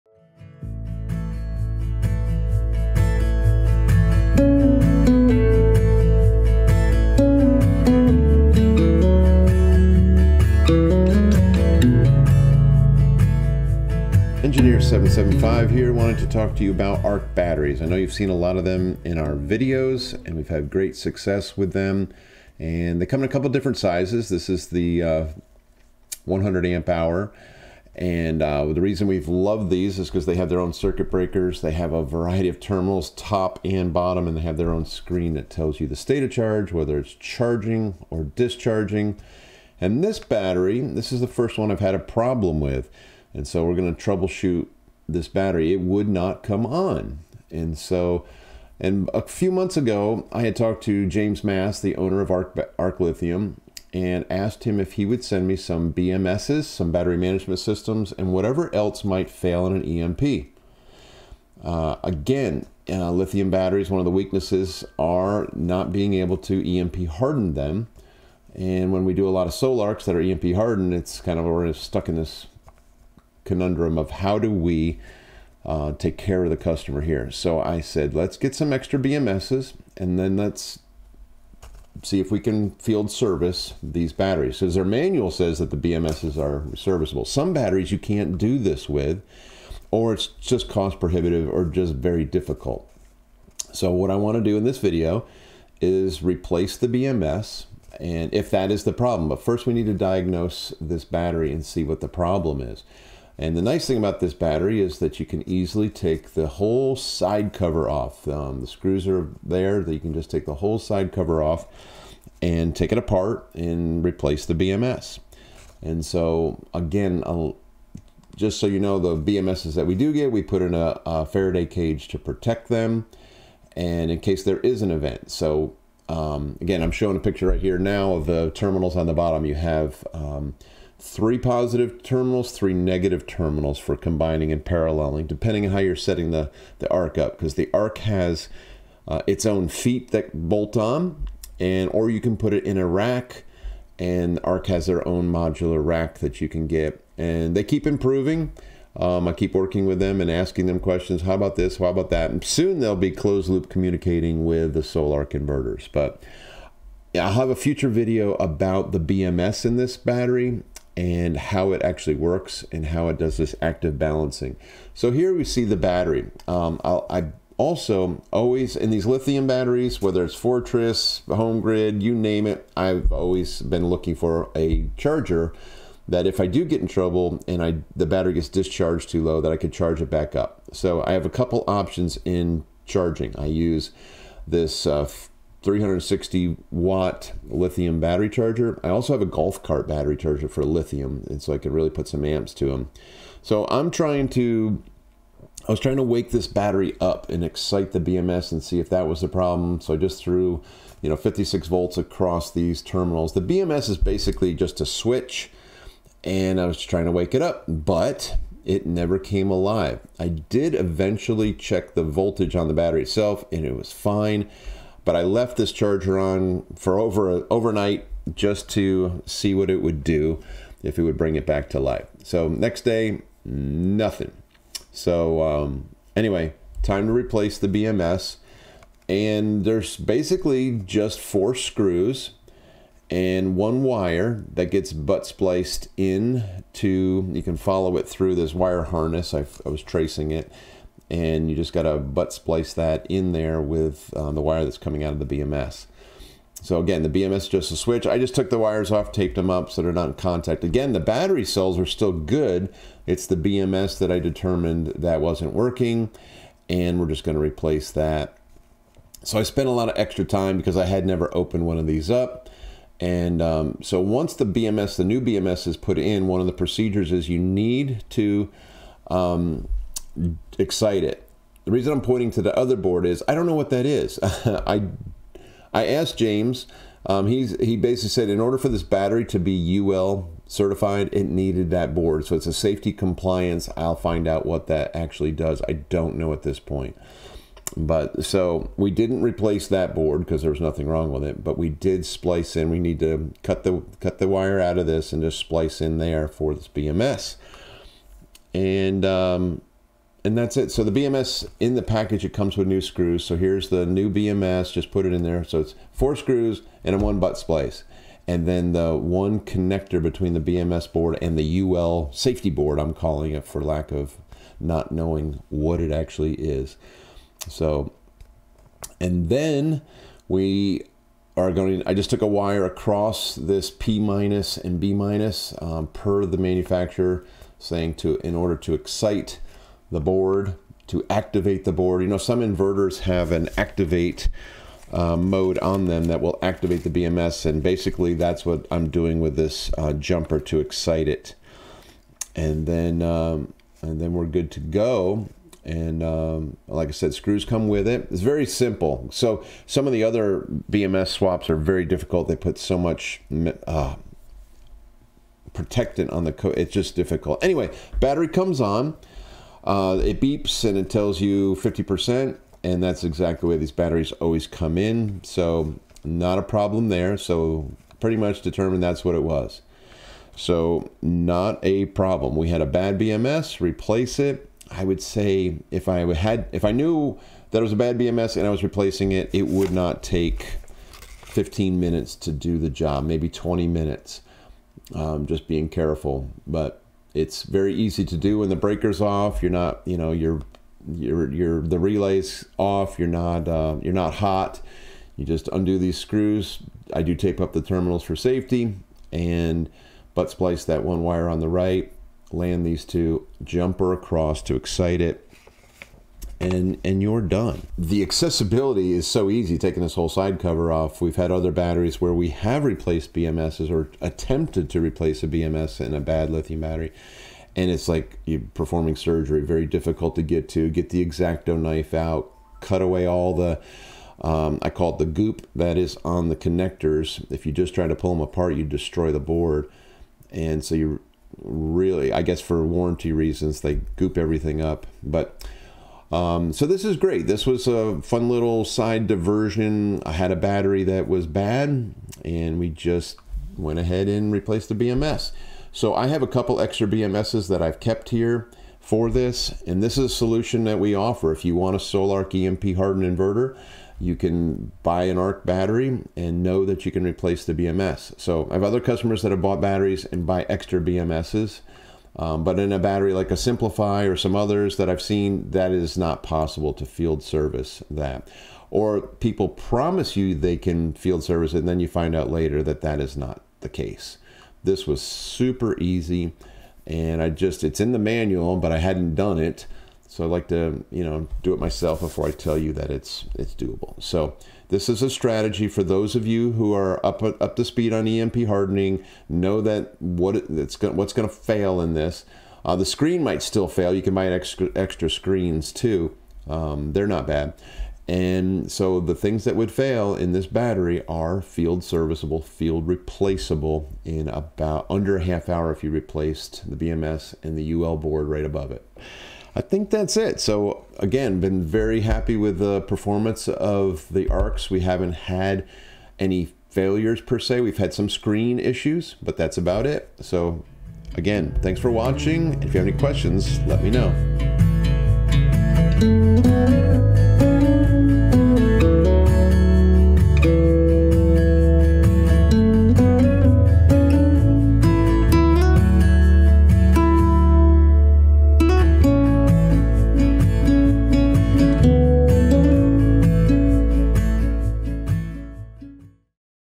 BMS Replacement | ARK Lithium
This ARK lithium battery stopped working so we opened her up to find the problem!

I also have a Rumble account!